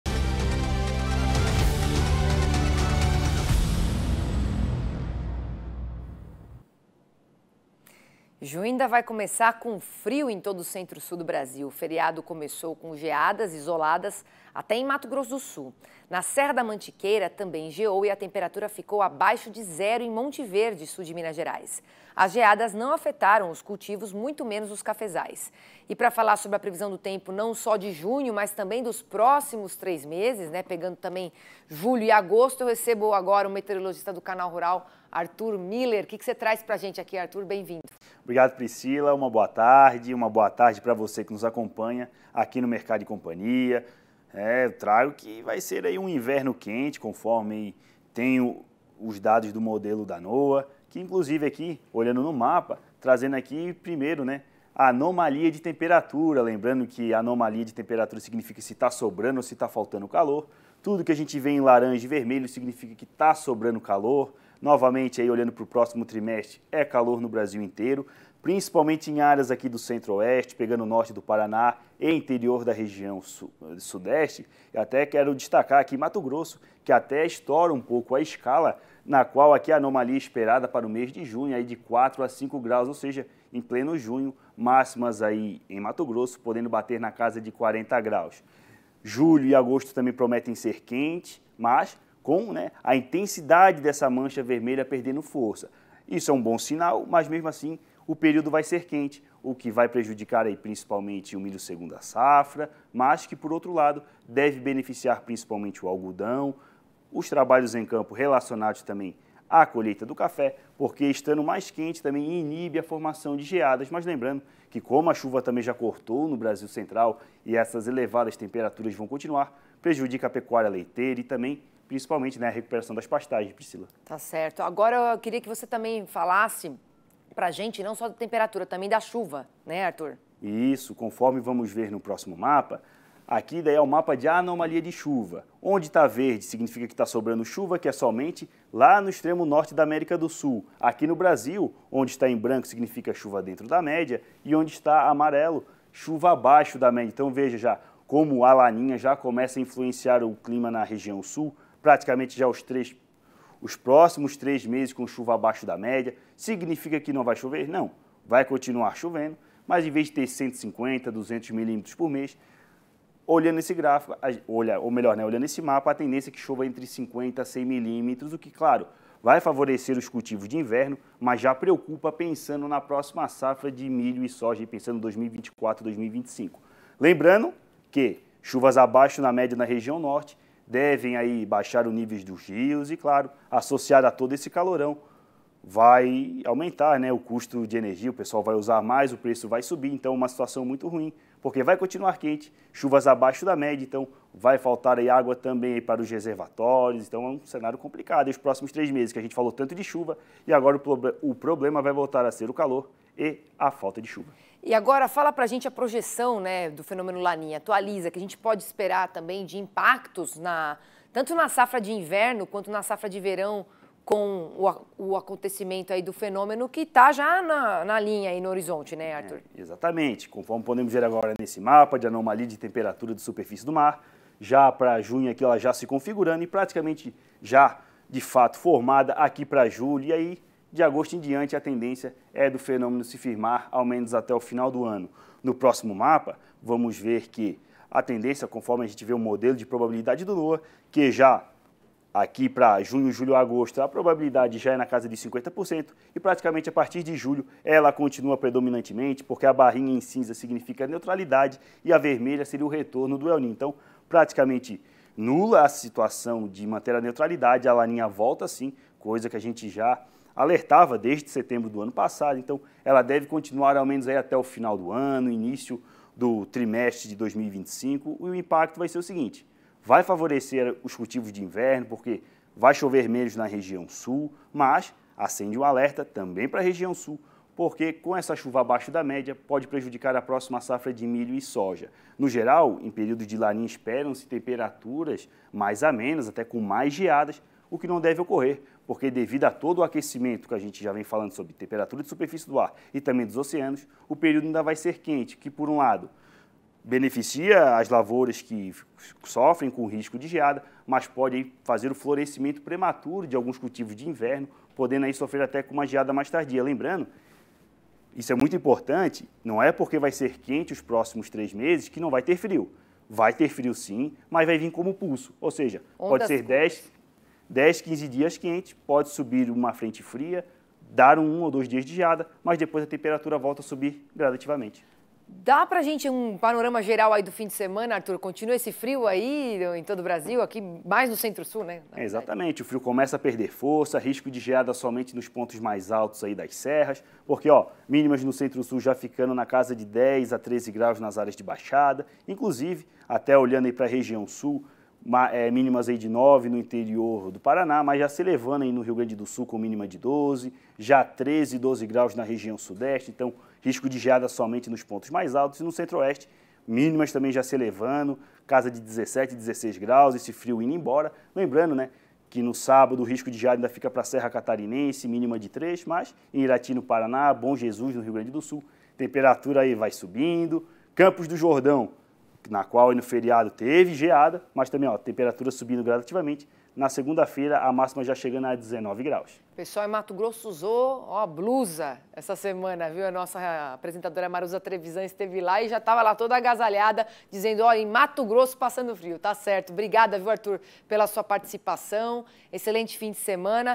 LA NIÑA: O QUE ESPERAR DE IMPACTOS PARA A PRODUÇÃO AGRÍCOLA?
Junho ainda vai começar com frio em todo o Centro-Sul do Brasil. O feriado começou com geadas isoladas até em Mato Grosso do Sul. Na Serra da Mantiqueira também geou e a temperatura ficou abaixo de zero em Monte Verde, sul de Minas Gerais. As geadas não afetaram os cultivos muito menos os cafezais.